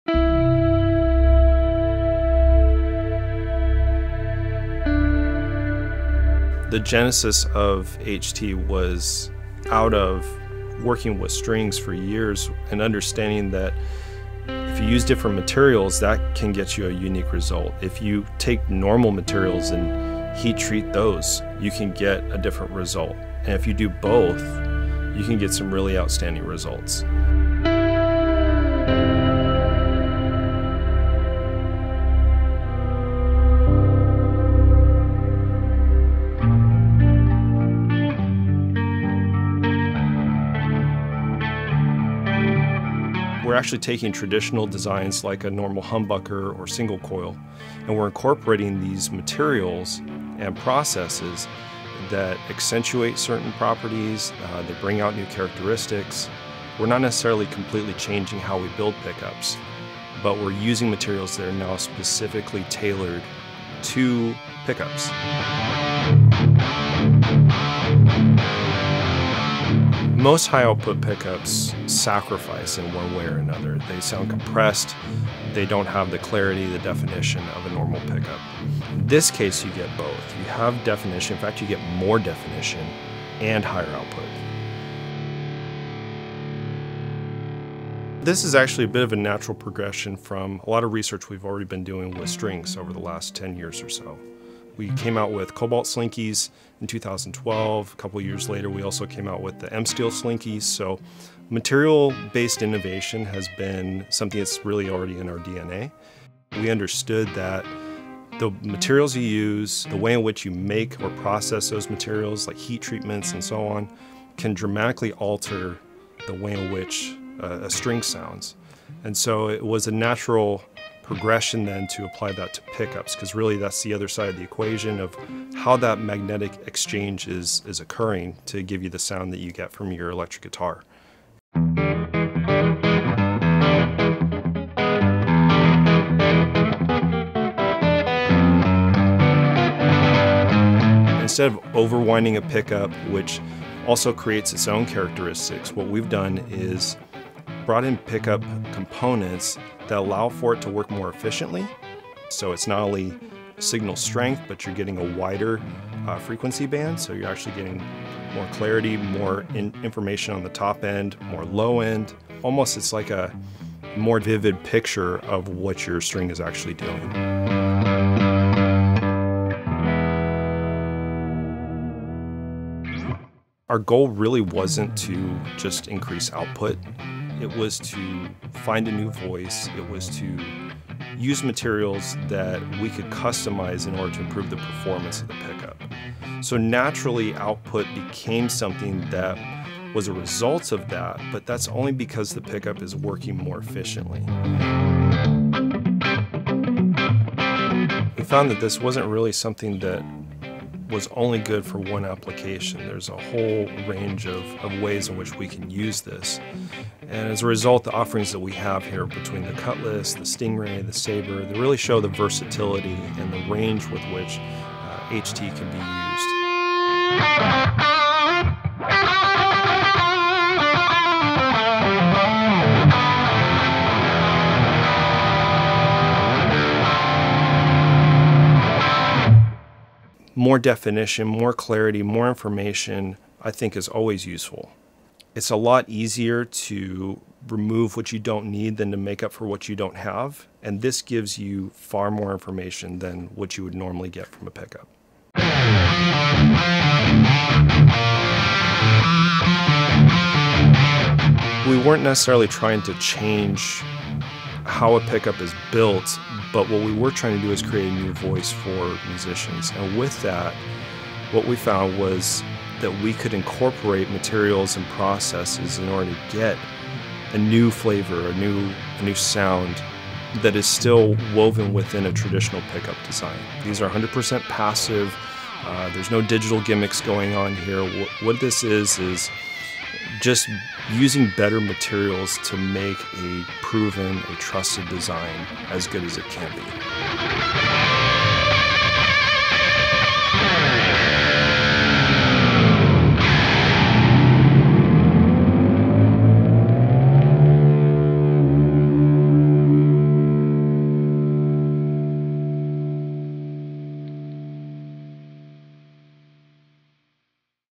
Ernie Ball Music Man: The HT Series
Inspired by over a decade of electric guitar string research, Ernie Ball has incorporated the principles and insights that contributed to the development of Cobalt and M-Steel Slinky, resulting in new material-based innovation for pickups. This all-new patent-pending HT (Heat Treated) pickup technology brings with it a new series of HT-equipped instruments, including a Cutlass HT, StingRay HT, and a Sabre HT.

HT (Heat Treated) pickups are able to achieve an ultra-high-output, powerful low-end response while retaining a distinctively clean, clear tone and definition at lower volume levels. These pickups utilize heat-treated pole pieces with a large ceramic magnet and an overwound coil that gives each pickup higher frequency harmonics and excellent touch sensitivity. The Cutlass HT and StringRay HT are available in Showtime, Brûlée, Midnight Rider, and Raspberry Burst. The Sabre HT is available in Yucatan Blue, Raspberry Burst, Showtime, and Snowy Night. The HT series is in stock and shipping now!